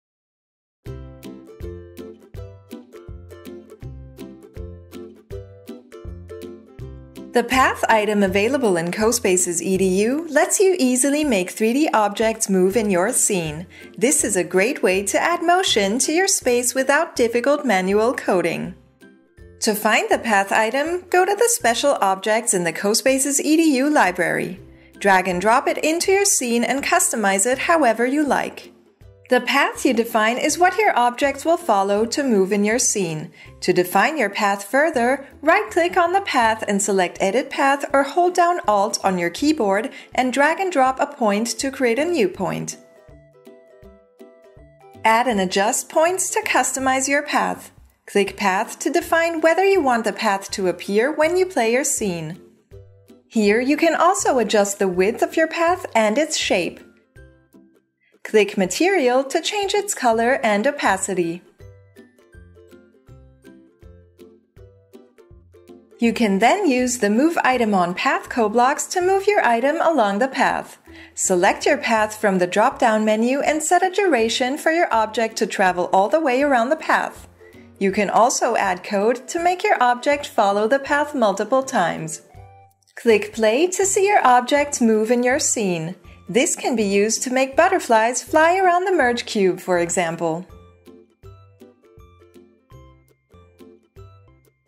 The Path item - CoSpaces Edu
This tutorial shows how to use the path item in CoSpaces Edu to easily make objects move in your scene and add motion to your creations!